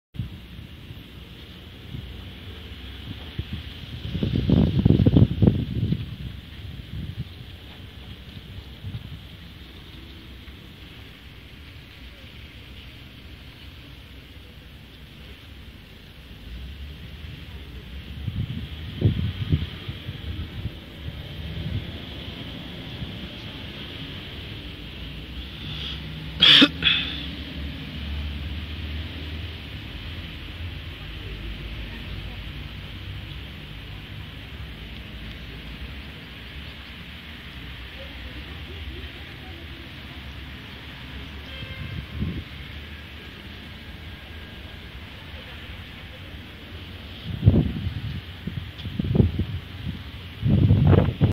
Lenovo A830 HD пример записи видео
Пример видеозаписи со смартфона Lenovo a830  в формате HD (1280*720).